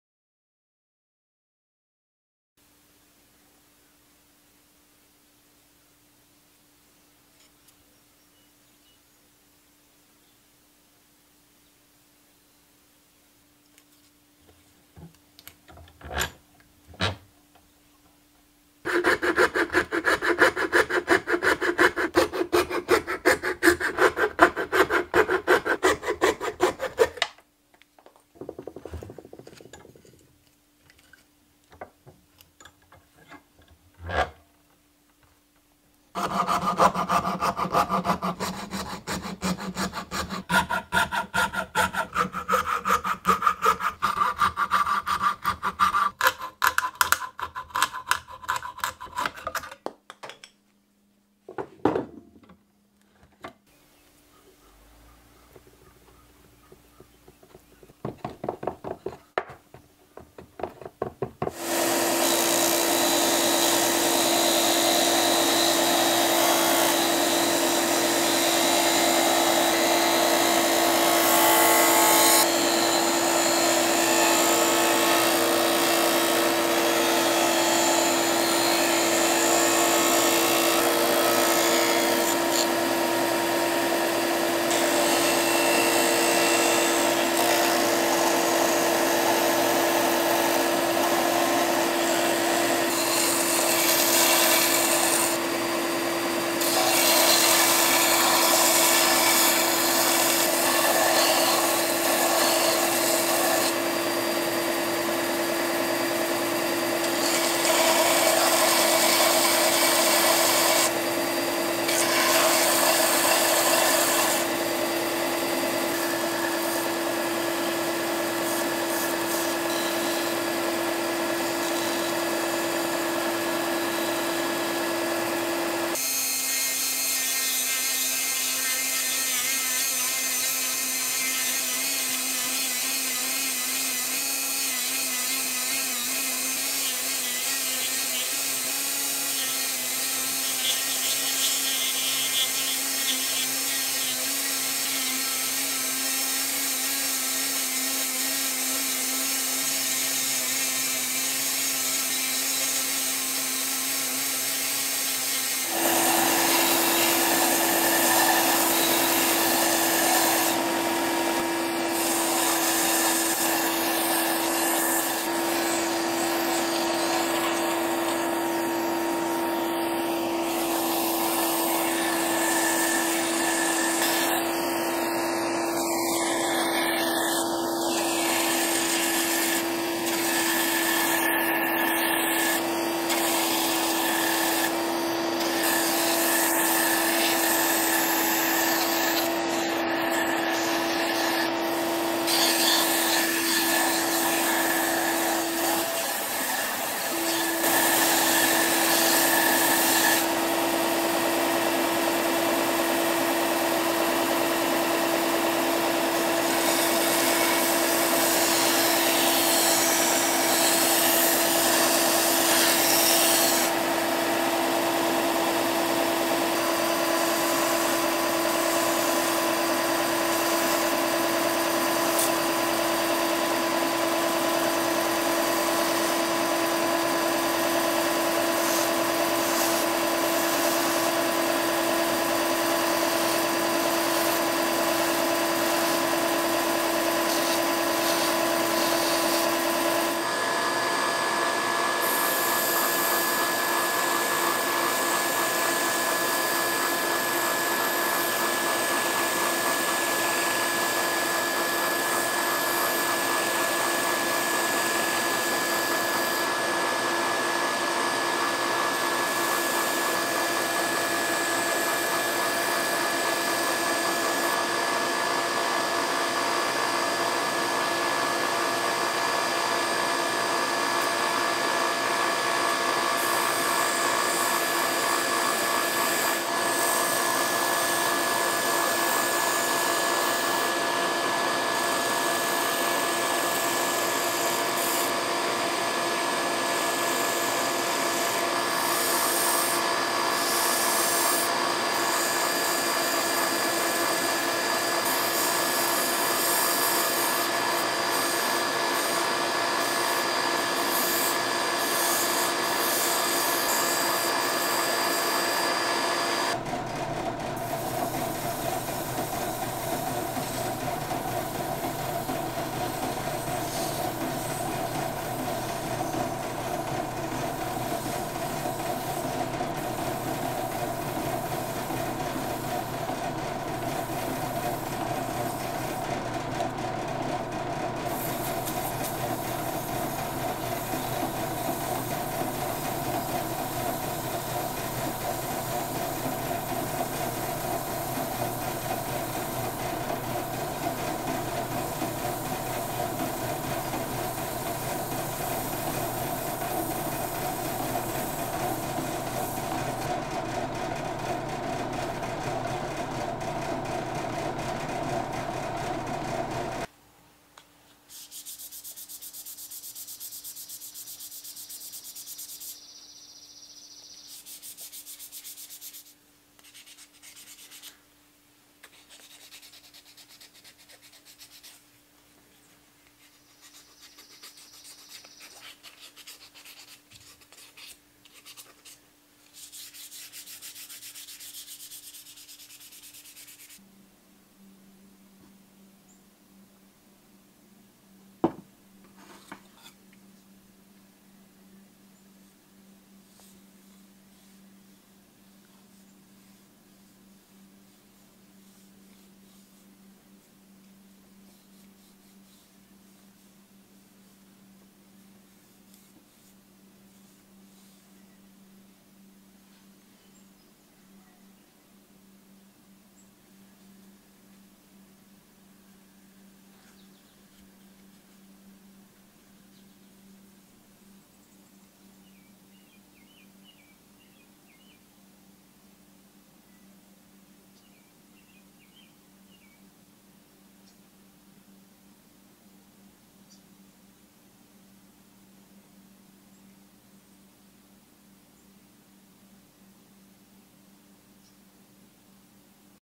Making a Chef's Knife from BIRD'S EYE Maple
I take a break from lure making to craft a decorative chef's knife from bird's eye maple. Carved from a piece of firewood headed for the furnace, this knife reveals the beauty of bird's eye that was hidden inside.